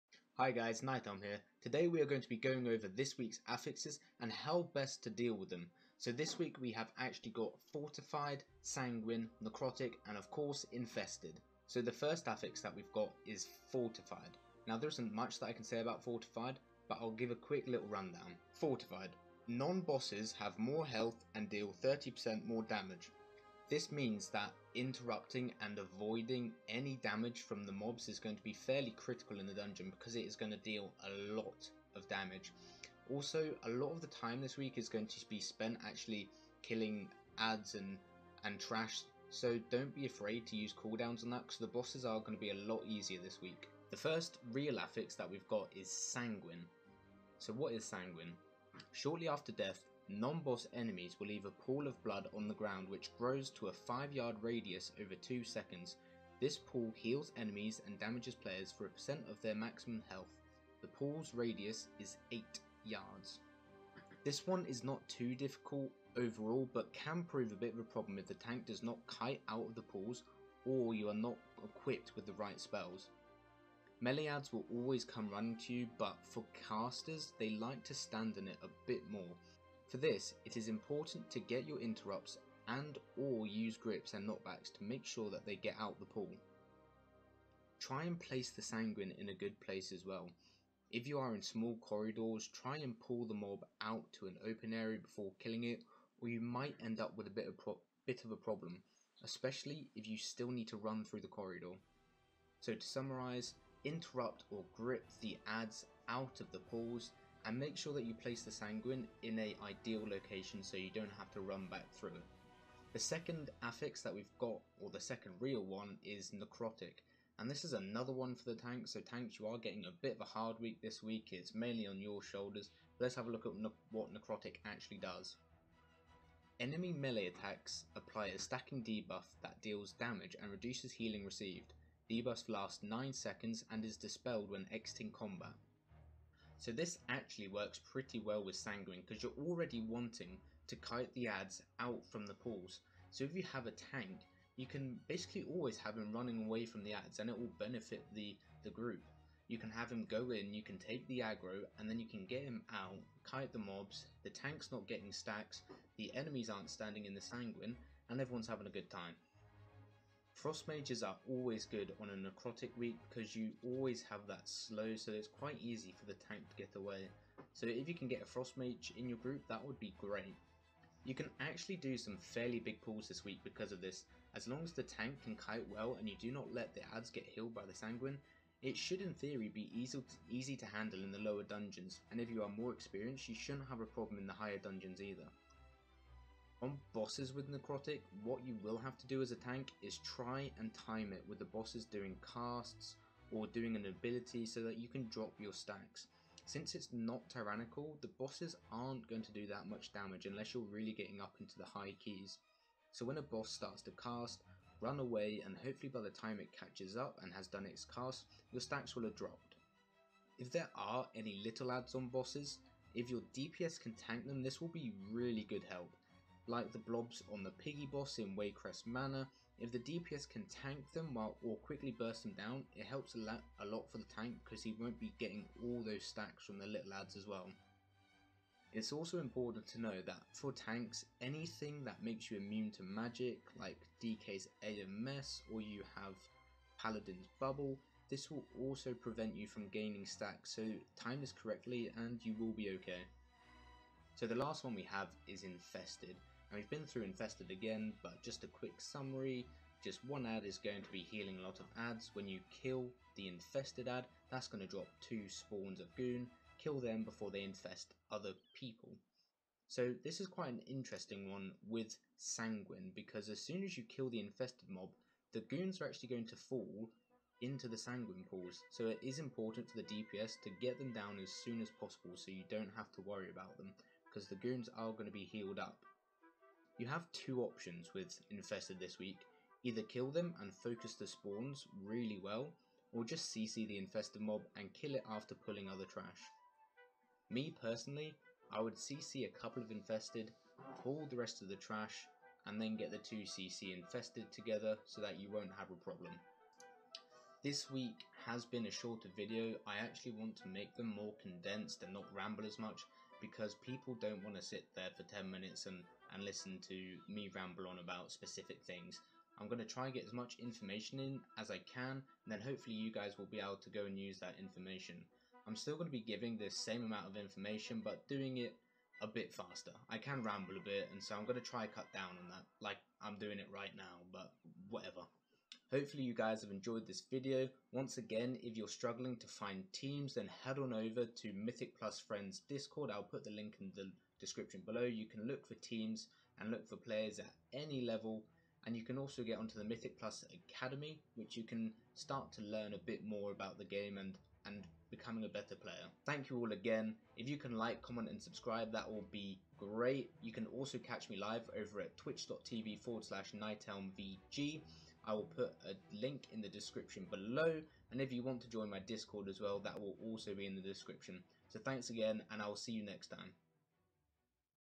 Weekly Affix Guide - Fortified, Sanguine, Necrotic
In this guide I hope to give some advice on how to best handle the affixes for the week. This week it is very much on the tank's shoulders to help the group get through the dungeon!

If you want to catch me live feel free to follow me over at: